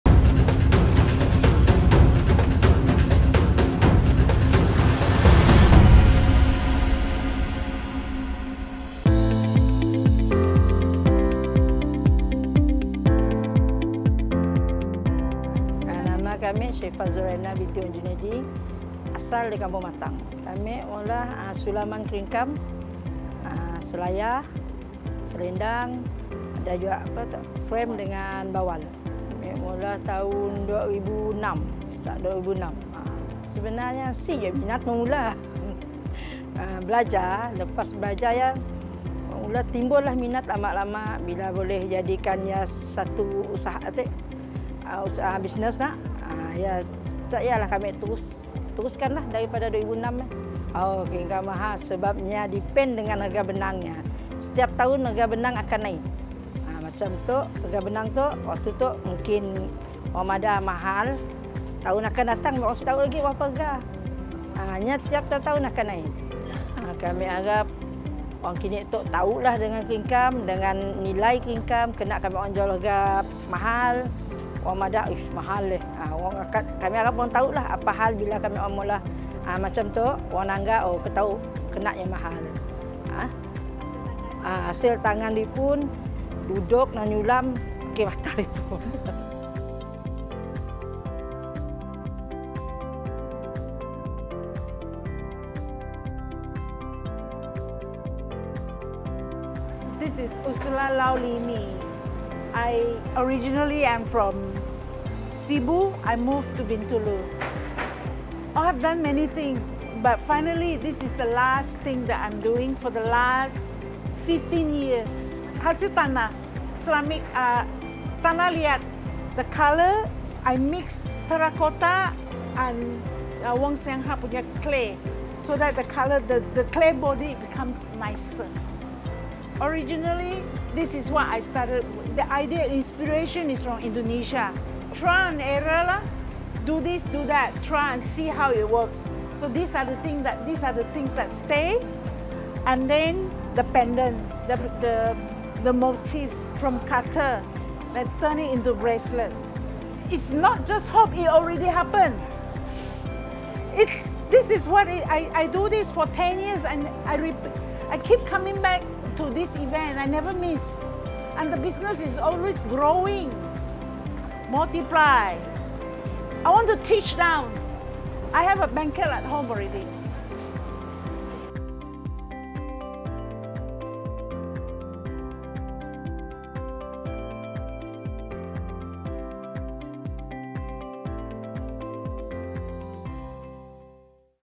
Demonstrasi & Jualan Kraf Part 6 | Festival Kraf Borneo Sarawak 2024
Usahawan-usahawan yang terlibat dalam Festival Kraf Borneo (FKB) Sarawak yang diadakan sempena Kuching Waterfront Festival 2024.